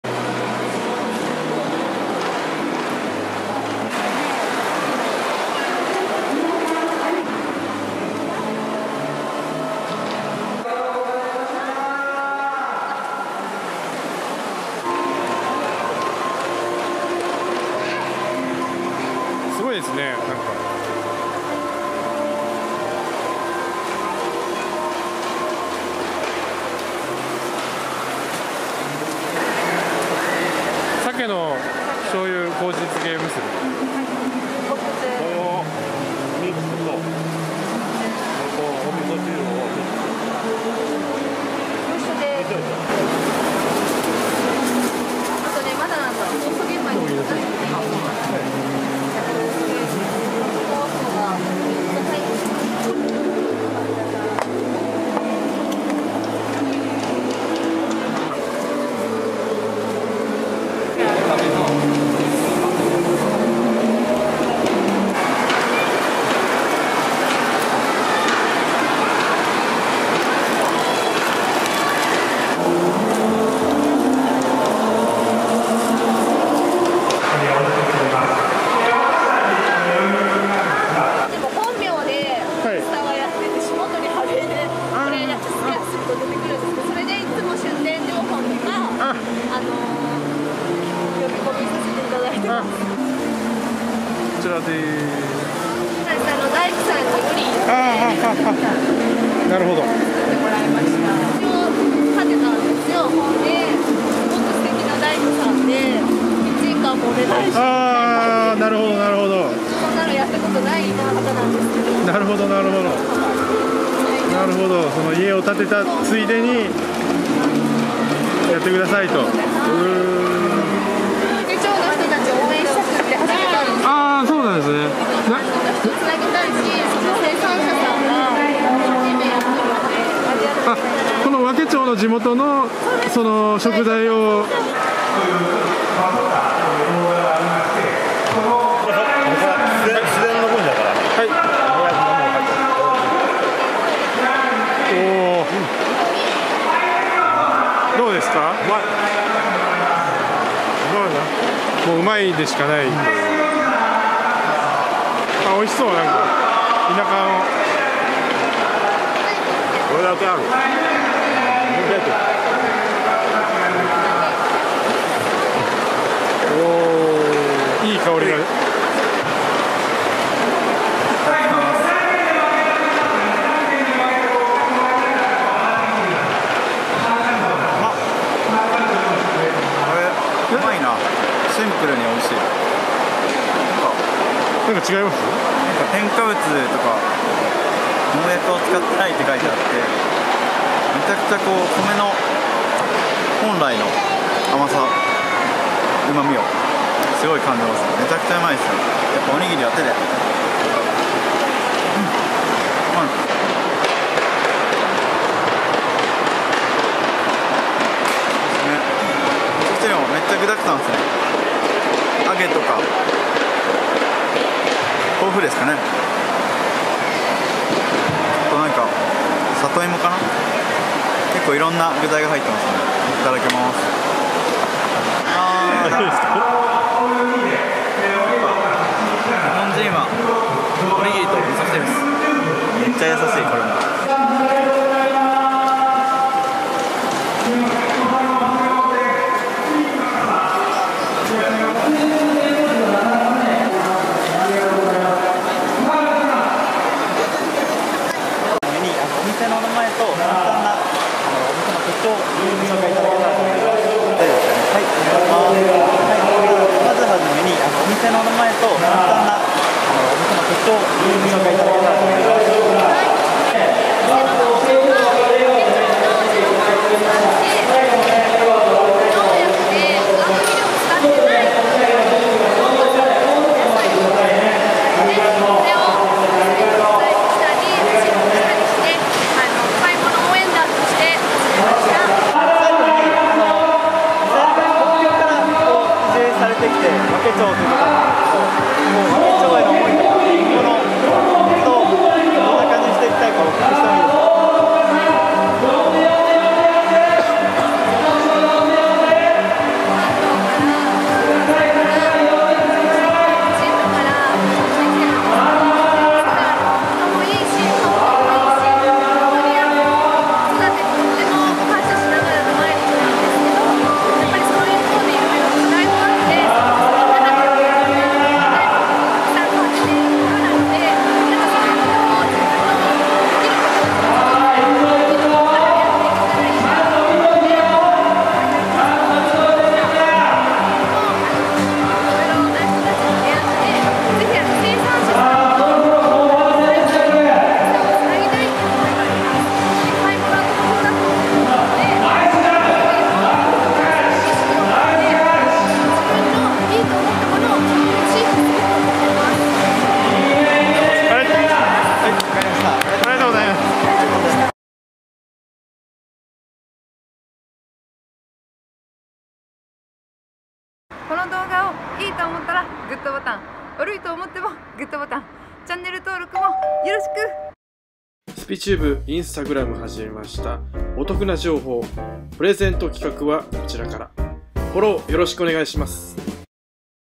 【岡山県和気町に移住して来てのキッチンカー】美味しいおむすび＆具だくさんのお味噌汁！美味しい！
再生リスト